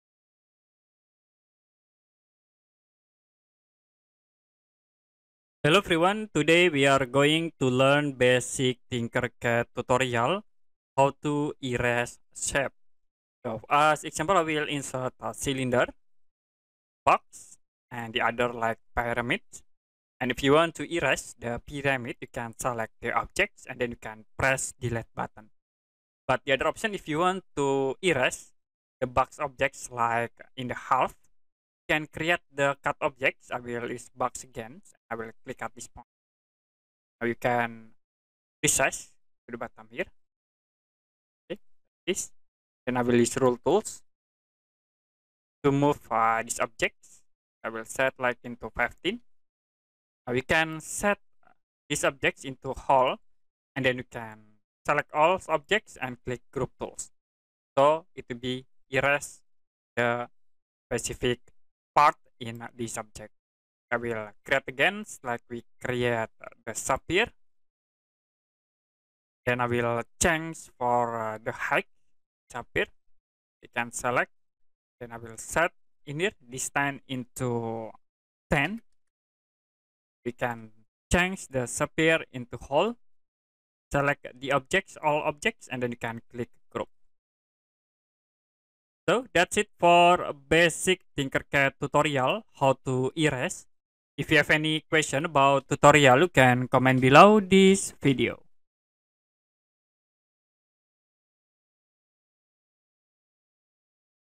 TinkerCAD How To Erase
Looking for a tutorial on how to erase in TinkerCAD? Learn how to use this feature effectively to edit and refine your 3D designs with ease.